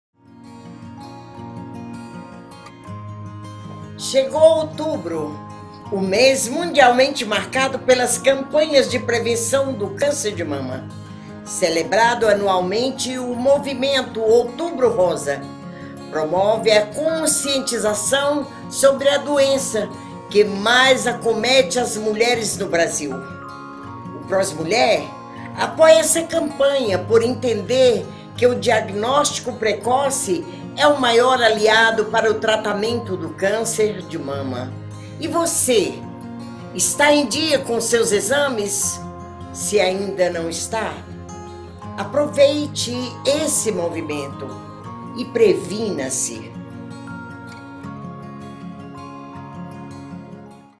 PROS MULHER - OUTUBRO ROSA
Outubro Rosa é a campanha de conscientização que tem como objetivo principal alertar as mulheres e a sociedade sobre a importância da prevenção e do diagnóstico precoce do câncer de mama. Se diagnosticado na fase inicial tem até 95% de chance de cura.

Se toque, se cuide e se ame. Nós apoiamos essa causa! Faça o autoexame, previna-se!